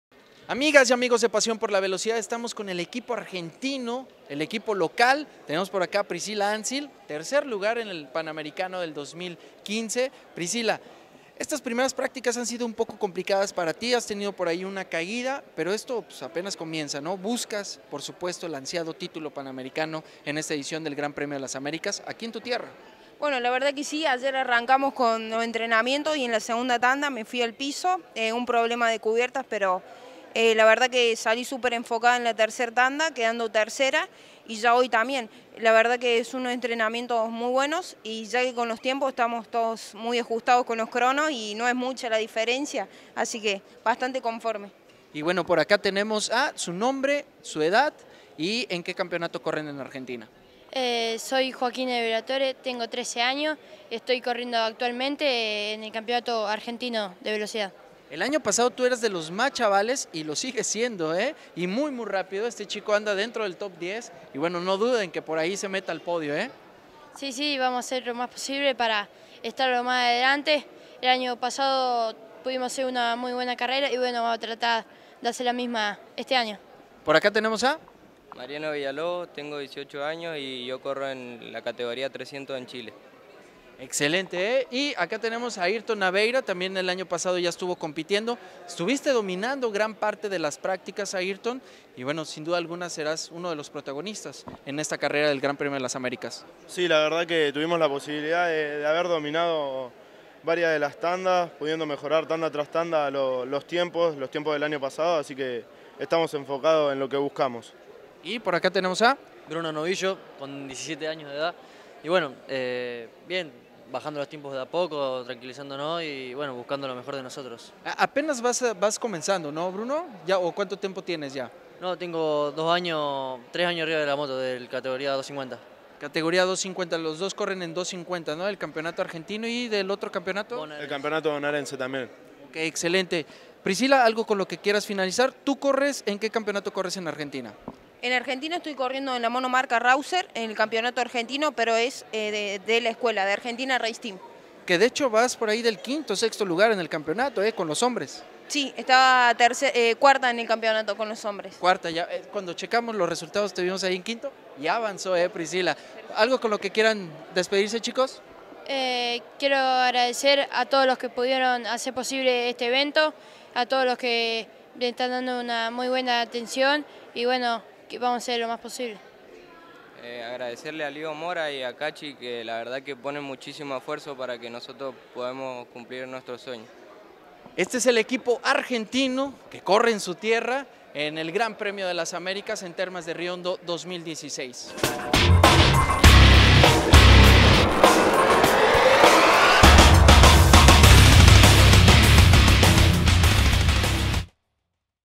Equipo Argentino en el Gran Premio de las Américas Panamericano de motociclismo 2016
Conoce algunos de los pilotos argentinos que tienen acción del 22 al 25 de septiembre en el campeonato Panamericano 2016 Gran Premio De Las Américas en el circuito de Termas de Río Hondo, provincia de Santiago del Estero, Argentina. Entrevista realizada el pasado viernes 23 de septiembre 2016. Una Producción de PXLV.